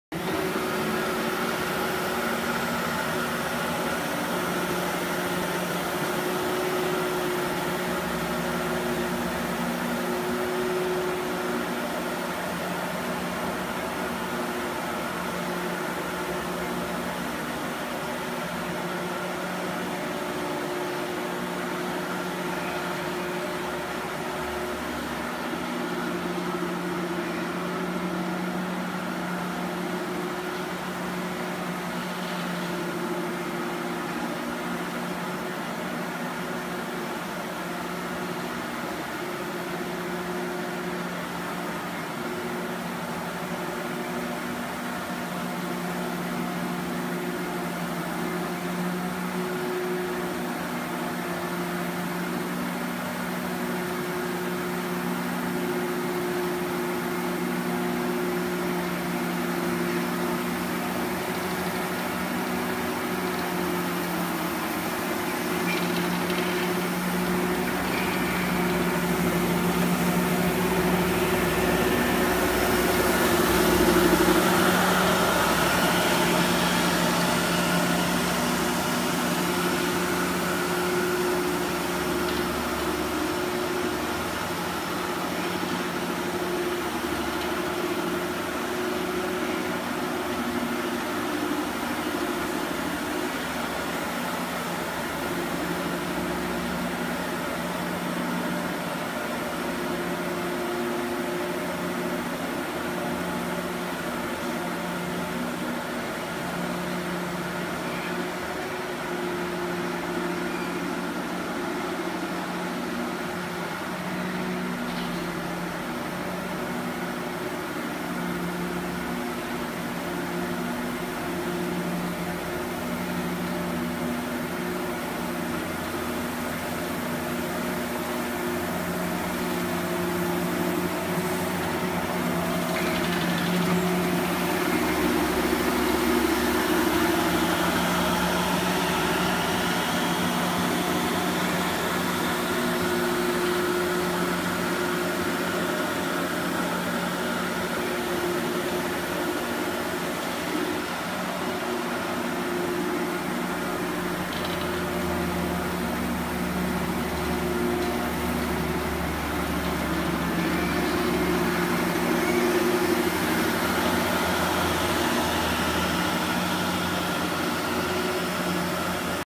Beta Solutions XR40 Ride On Scrubber Dryer Demo - Warehouse
Beta Solutions testing the Factory Cat XR40C ride on scrubber dryer after a routine service. It cleans this warehouse floor up easily.